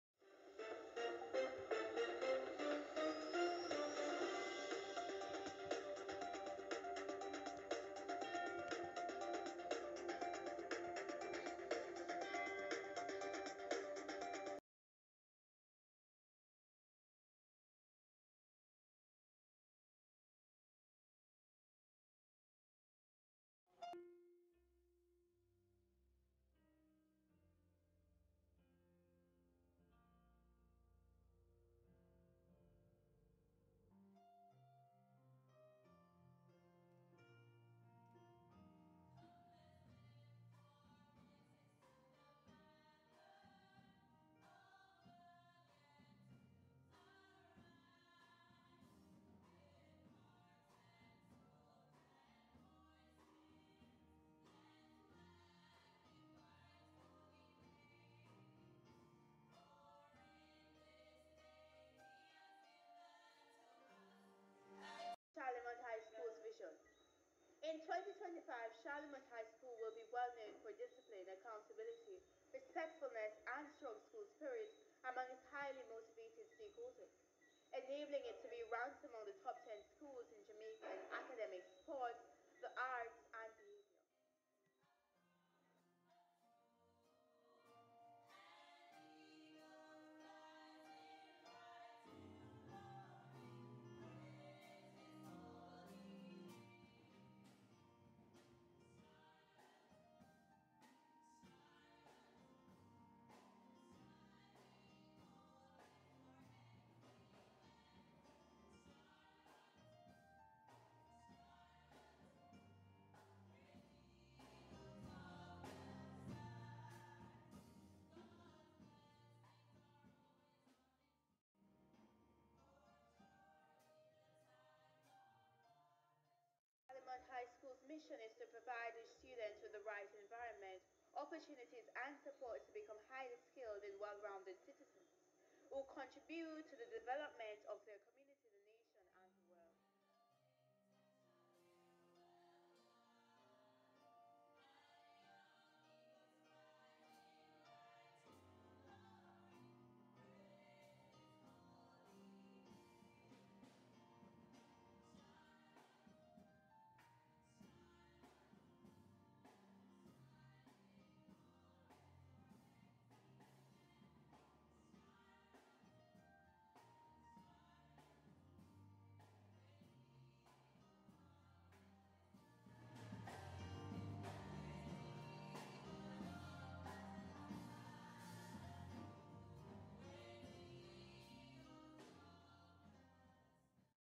The Charlemont High School -Achievements 2024
This year the Charlemont High School is Celebrating 45 years. We have accomplished a lot in the areas of academics, sports and the arts. We have also launched our digital ID system which will help us to effectively track students attendance.

Charlemont High School
Striving for Excellence!